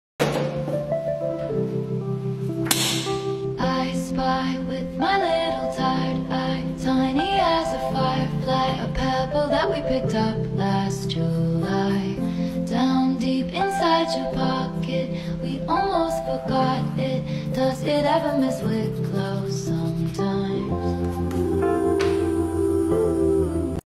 Today’s sketch on page23 🧡🐠💛🐟 Cute little clown fish and peas
What I use:
✏️ Faber-Castell Colour pencils
📓 Talens Art Creation Skechbook
✒️ Sakura Fineliner Pens
🎨 Daler-Rowney Acrylic Paint
📷 My camera - Canon EOS M50